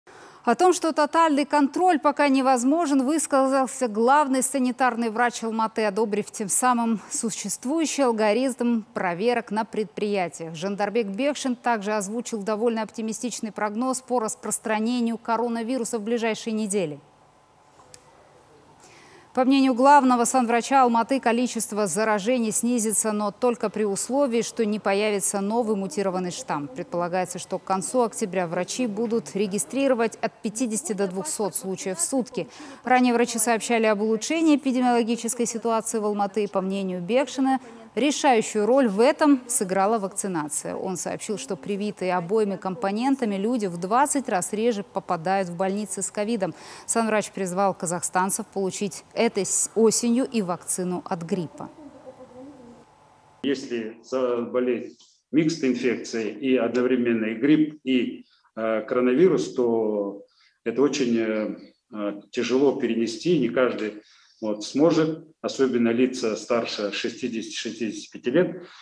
Жандарбек Бекшин озвучил оптимистичный прогноз по распространению COVID
О том, что тотальный контроль пока невозможен, высказался  главный санитарный врач Алматы. Одобрив тем самым существующий алгоритм проверок на предприятиях. Жандарбек Бекшин также озвучил довольно оптимистичный прогноз по распространению коронавируса в ближайшие недели.
По мнению главного санврача Алматы, количество заражений снизится, но только при условии, что не появится  новый мутировавший штамм.  Предполагается, что к концу октября врачи будут регистрировать от 50 до 200 случаев в сутки. Ранее врачи сообщали об улучшении эпиде- миологической ситуации в Алматы. По мнению Бекшина, решающую роль в этом сыграла вакцинация. Он сообщил, что привитые обоими компонентами люди в 20 раз  реже попадают в больницы  с ковидом. Санврач призвал казахстанцев получить этой осенью и вакцину от гриппа.
ЖАНДАРБЕК БЕКШИН, главный сантарный врач Алматы:
– Если заболеть микст-инфекцией, то есть, одновременно и гриппом, и коронавирусом, то это очень тяжело перенести, не каждый сможет, особенно лица старше 65 лет.